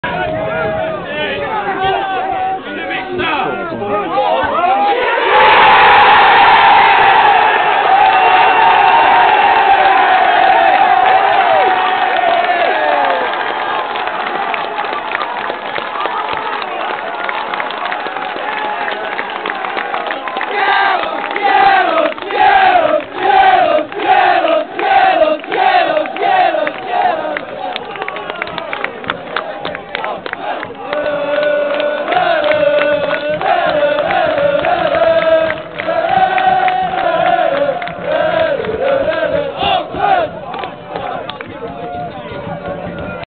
Joe Burnell heads in for Oxford against Lewes
Joe Burnell heads in for Oxford United against Lewes.